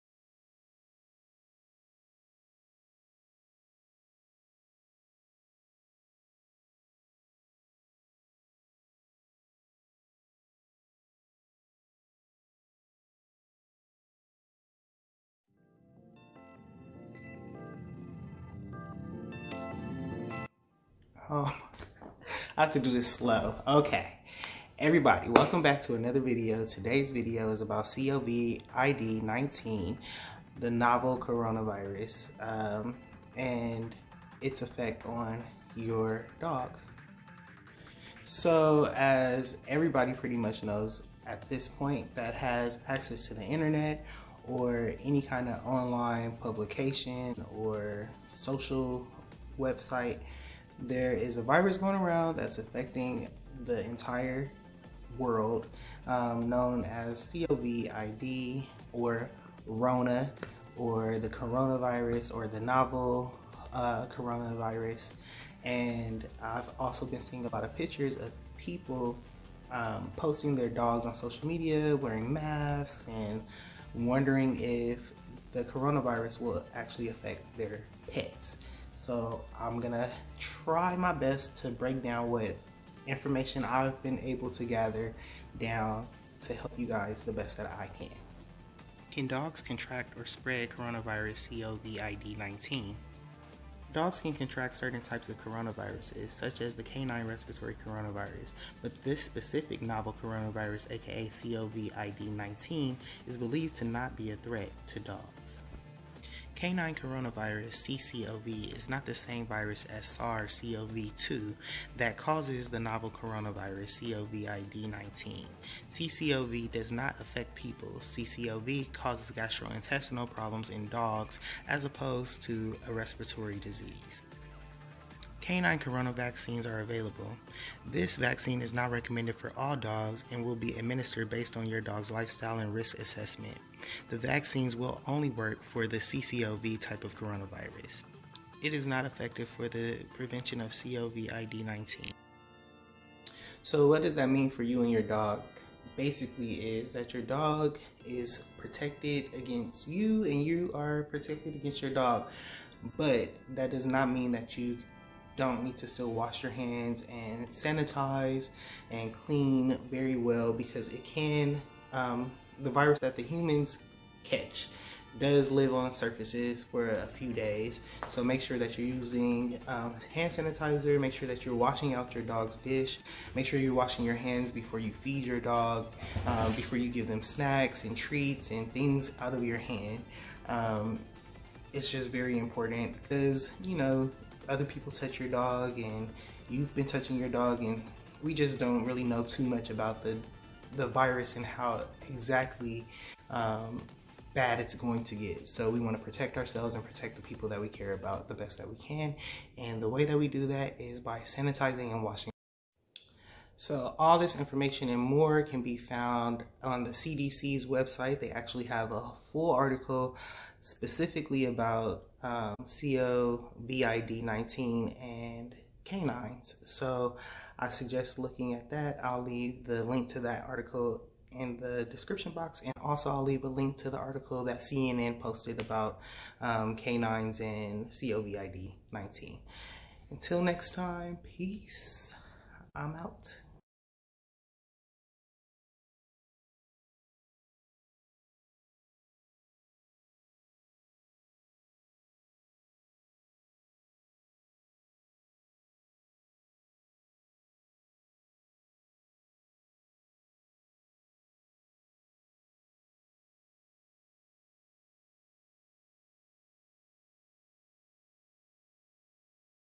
Life with Diddy: Can My Dog Catch Coronavirus?
I kept coming across pictures and videos on social media of people "protecting" their dogs against COVID-19. This made me do my own research and this is the information I found to help some of you that are worried about your dogs being affected.